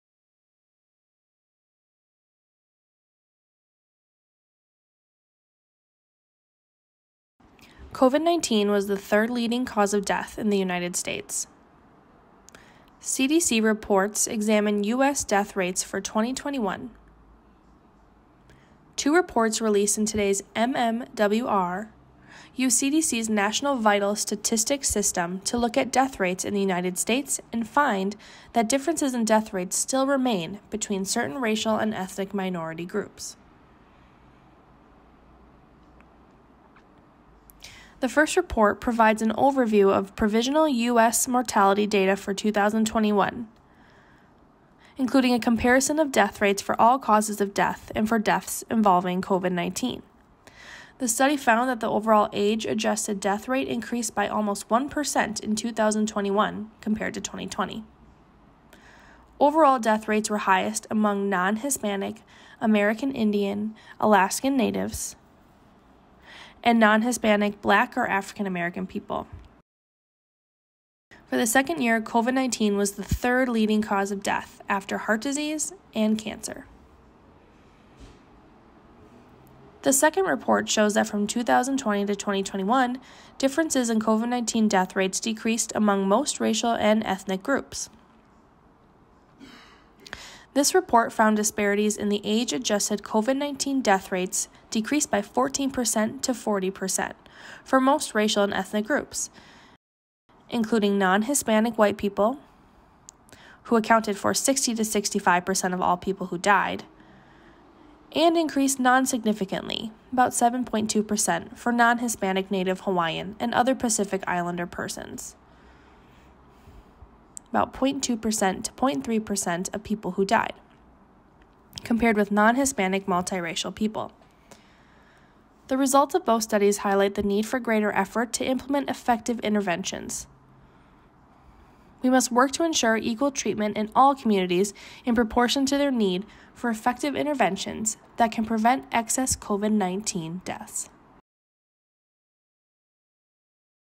COVID-19 Was Third Leading Cause of Death in U.S.
CDC reports examine U.S. Death Rates for 2021
Two reports released in today’s MMWR use CDC’s National Vital Statistics System to look at death rates in the United States and find that differences in death rates still remain between certain racial and ethnic minority groups.
The first report provides an overview of provisional U.S. mortality data for 2021, including a comparison of death rates for all causes of death and for deaths involving COVID-19. The study found that the overall age-adjusted death rate increased by almost 1% in 2021 from 2020. Overall death rates were highest among non-Hispanic American Indian/Alaskan Native and non-Hispanic Black or African American people. For the second year, COVID-19 was the third leading cause of death after heart disease and cancer.
The second report shows that from 2020 to 2021, differences in COVID-19 death rates decreased among most racial and ethnic groups. This report found disparities in the age-adjusted COVID-19 death rates decreased by 14%–40% for most racial and ethnic groups, including non-Hispanic White people, who accounted for 60%–65% of all people who died; and increased non-significantly (7.2%) for non-Hispanic Native Hawaiian and other Pacific Islander persons (0.2%–0.3% of people who died) compared with non-Hispanic multiracial people.
The results of both studies highlight the need for greater effort to implement effective interventions. We must work to ensure equal treatment in all communities in proportion to their need for effective interventions that can prevent excess COVID-19 deaths.